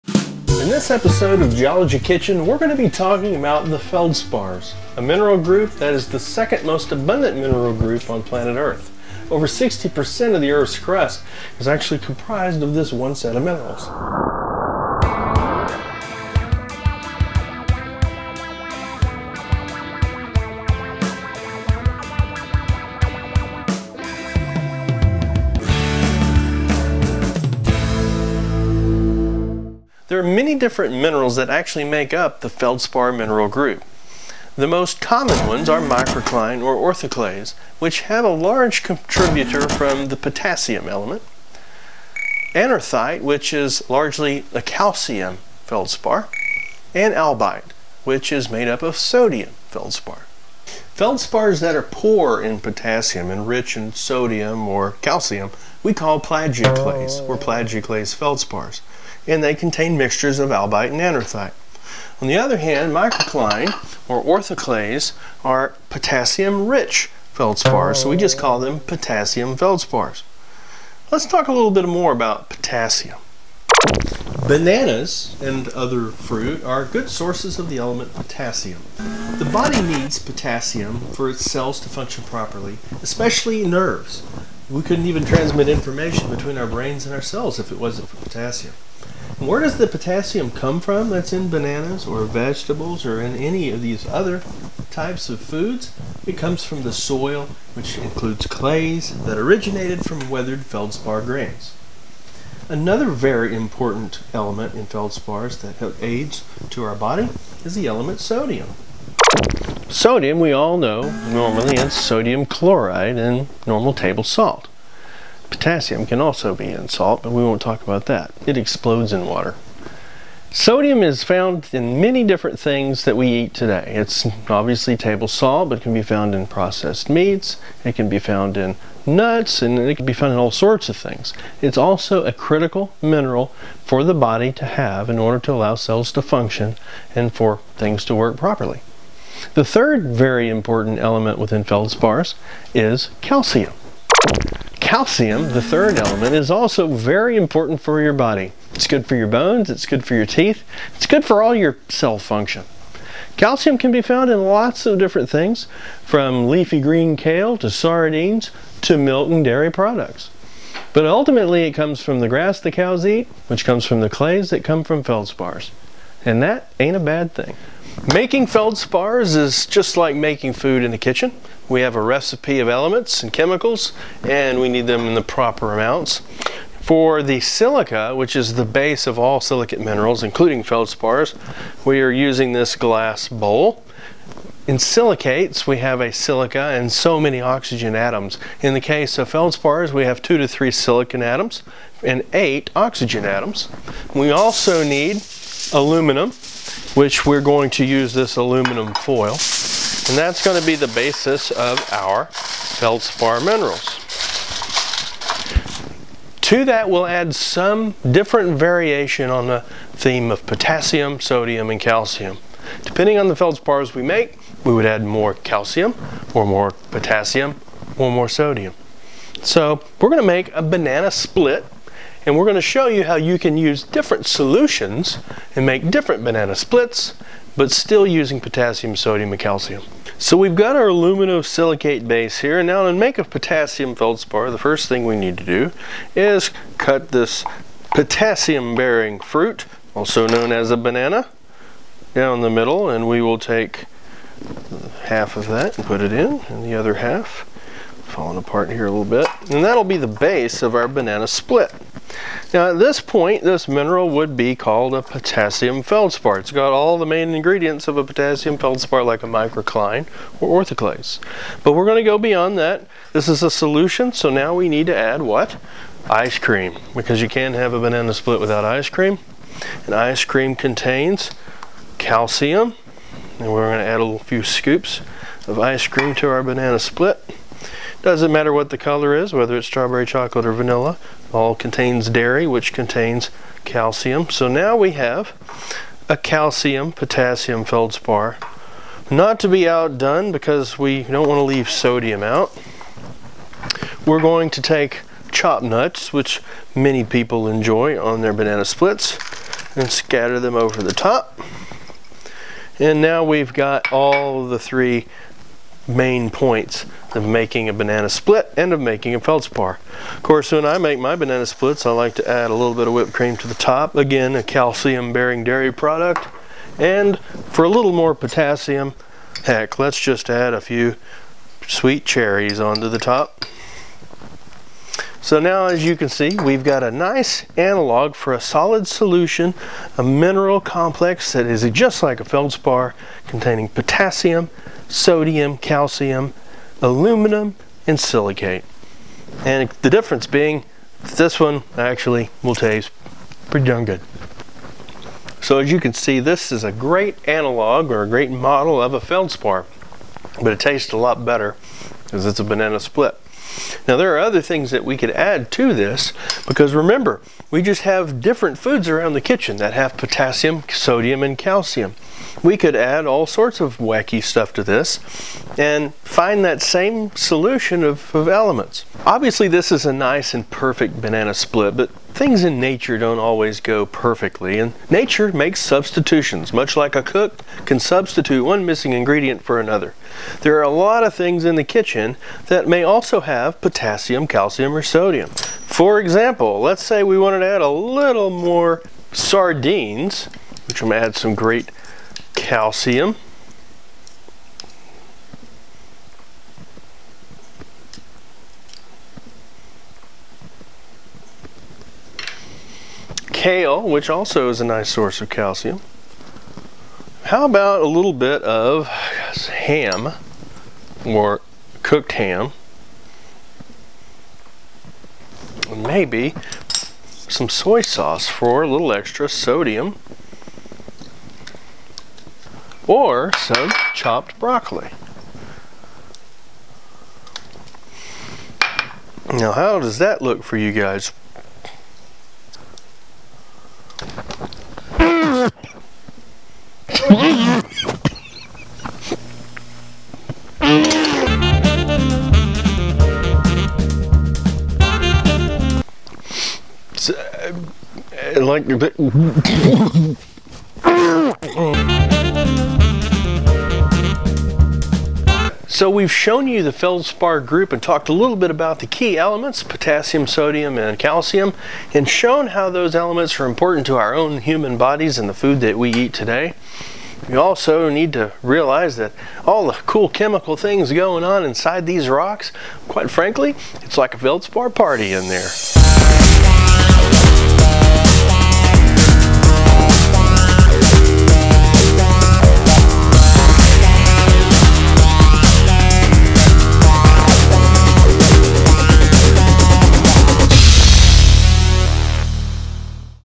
Geology Kitchen #13 - Feldpsarty
Feldspars are the second most common group of minerals on Earth.  The are important contributors to soils as they weather relatively easily to form clays.  Potassium, Sodium and Calcium are the most common elements in Feldspars passed on to plants and animals, and are all elements critical to human life.